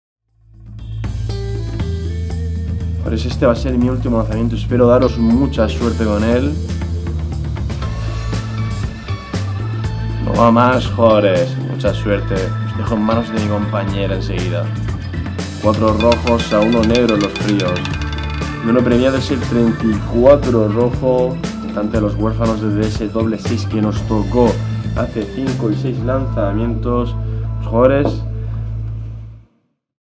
Ruleta en Vivo - Casino Online | Luckia
La Ruleta en Vivo de Evolution, una de las mejores Ruletas Live en el Casino Online de Luckia.

La finalidad del juego es acertar el número en el que parará la bolita mientras el croupier en vivo 24 horas reparte el juego.